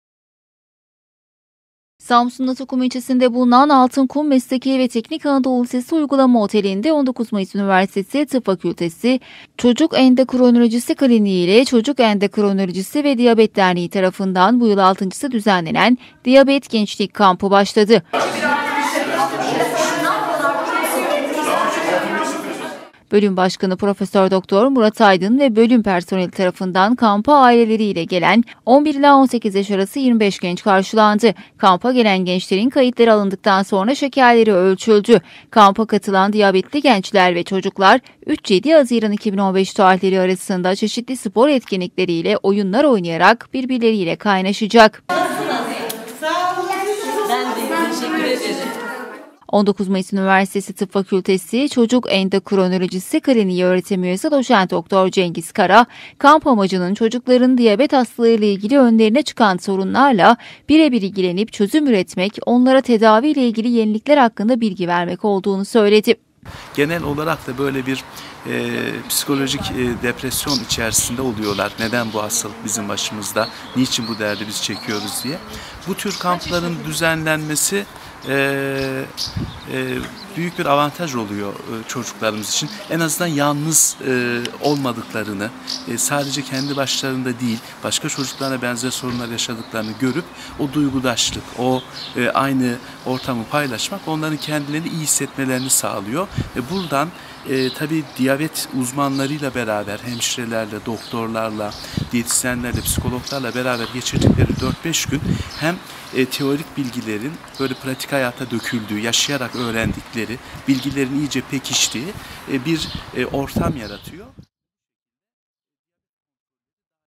OMÜ'DE DİYABET KAMPI
Samsun'da Ondokuz Mayıs Üniversitesi (OMÜ) Tıp Fakültesi Çocuk Endokrinolojisi Kliğini ile Çocuk Endokrinolojisi ve Diyabet Derneği tarafından, 25 diyabetli hasta öğrencinin katılımıyla, 'Diyabet Gençlik Kampı' başladı.